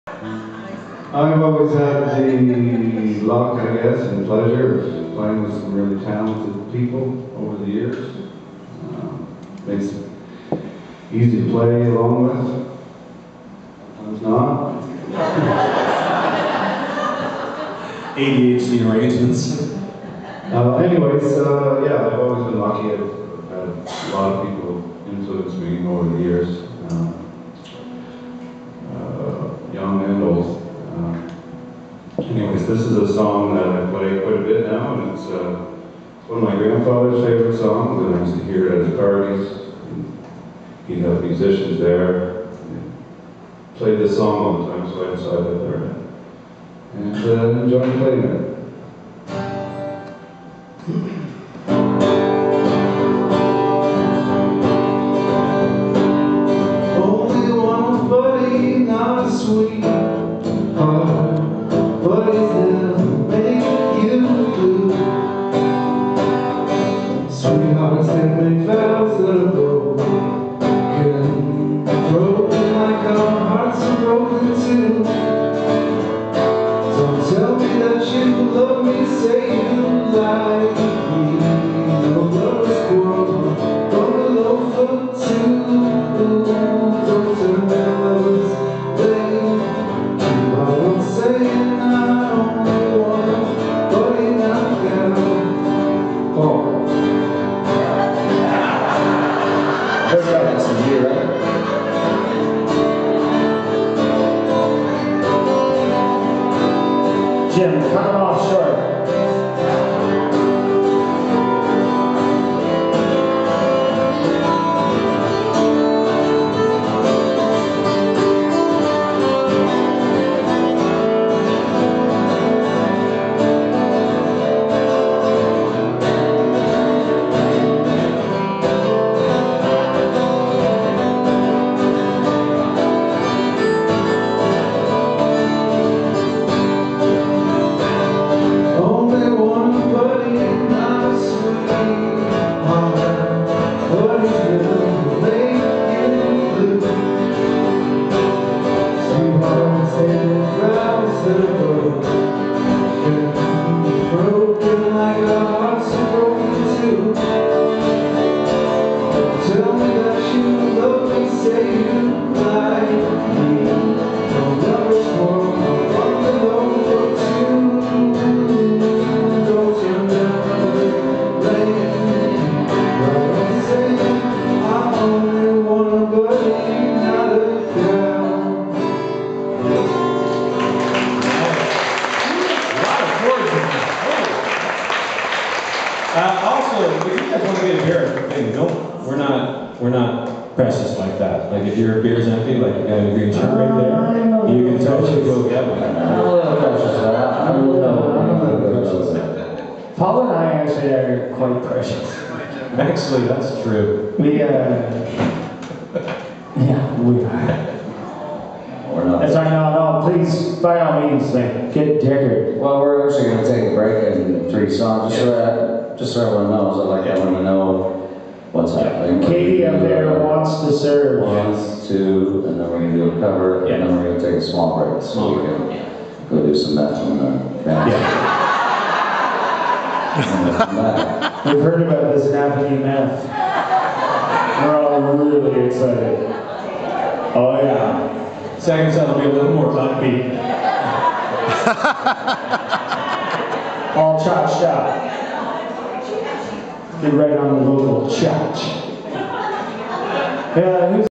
The Campfire Liars Club - August 24th 2018 Doghouse Studios
The Campfire Liars Club - August 24th 2018 Doghouse Studios in Napanee Ontario Featuring Paul Langlois (of The Tragically Hip), Greg Ball, Jim Tidman and Joe Carscallen.